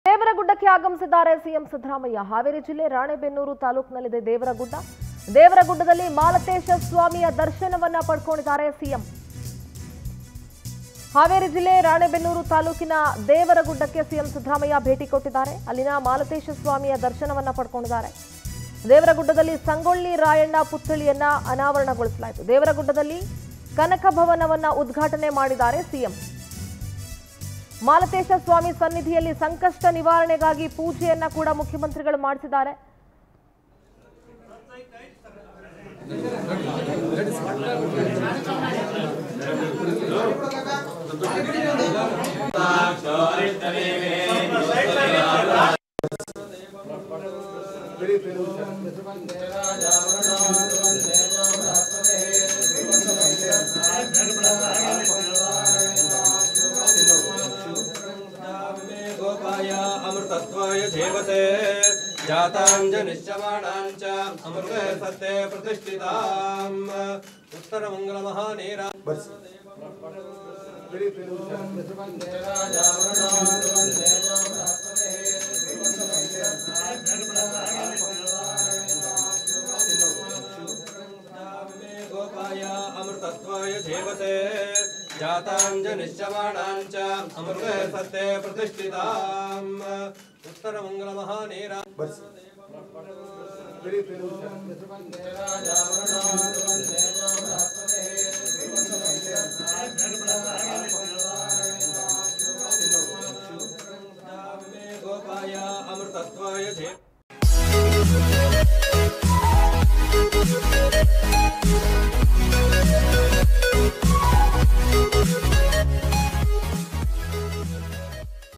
CM Siddaramaiah : ದೇವರಗುಡ್ಡಕ್ಕೆ ಆಗಮಿಸಿ ಮಾಲತೇಶ ಸ್ವಾಮಿಯ ದರ್ಶನ ಪಡೆದ ಸಿಎಂ ಸಿದ್ದರಾಮಯ್ಯ! | Haveri|Power Tv
CM Siddaramaiah : ದೇವರಗುಡ್ಡಕ್ಕೆ ಆಗಮಿಸಿ ಮಾಲತೇಶ ಸ್ವಾಮಿಯ ದರ್ಶನ ಪಡೆದ ಸಿಎಂ ಸಿದ್ದರಾಮಯ್ಯ! | Haveri | Power Tv News